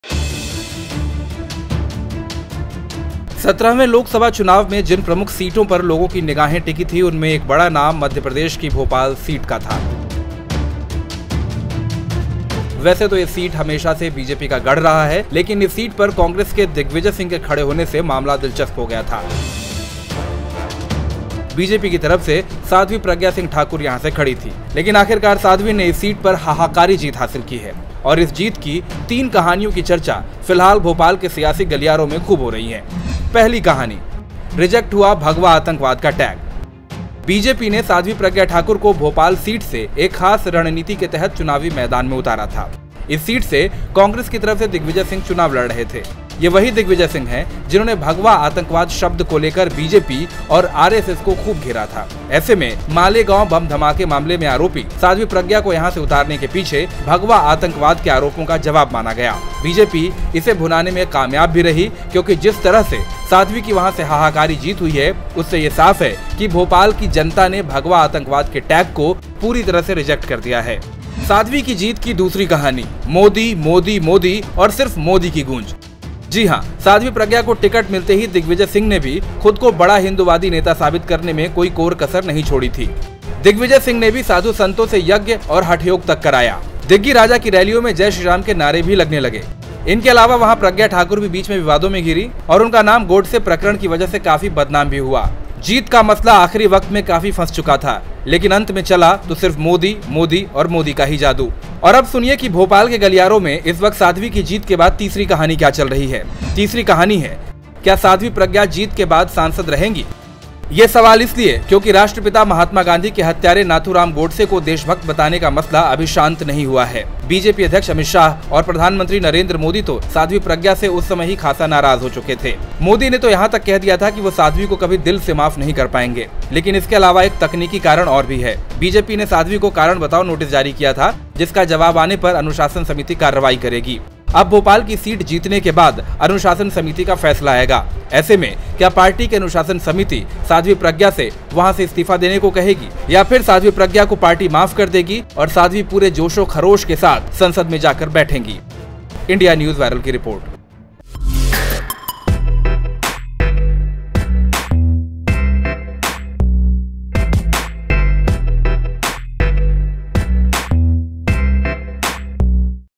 MP की भोपाल सीट पर साध्वी प्रज्ञा ठाकुर की जीत क्यों खास हैshah
MP की भोपाल सीट पर साध्वी प्रज्ञा ठाकुर की जीत क्यों खास है ? साध्वी की हाहाकारी जीत की 3 बड़ी कहानी जानने के लिए देखें रिपोर्ट